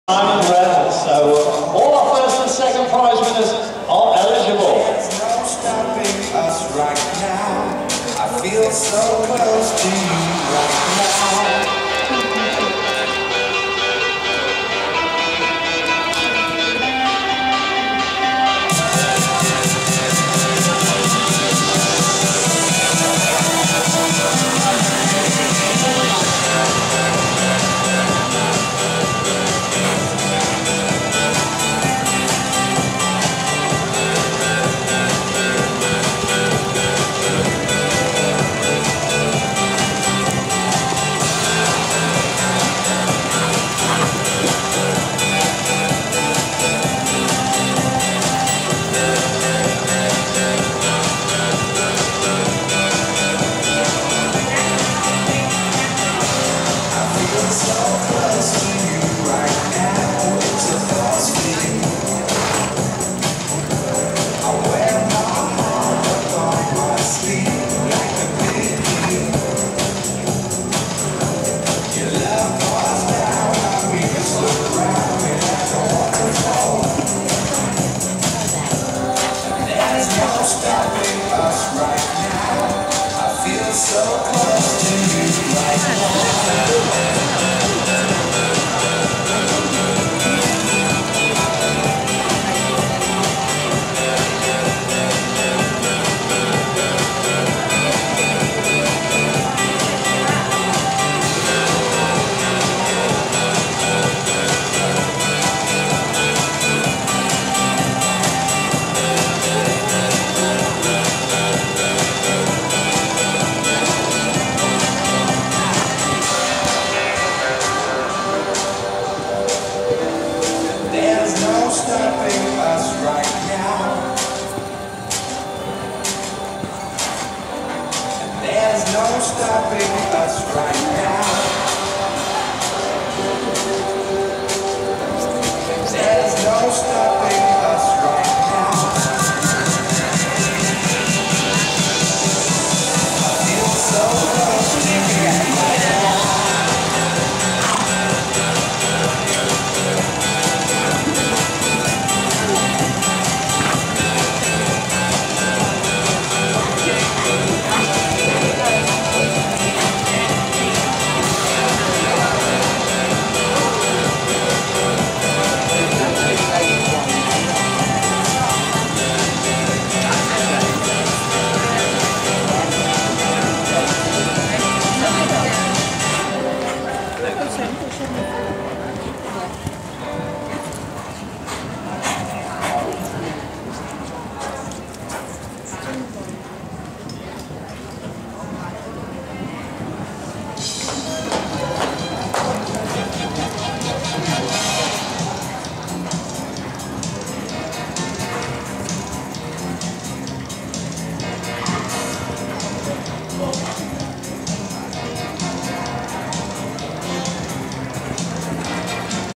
Clarendon Coloured Open Pony Of The Year Championship CHAPS 2012
Clarendon Coloured Open Pony Of The Year Championship CHAPS 2012 featuring Balkly Mardi Gras & 13 other ponies from the Open Finals.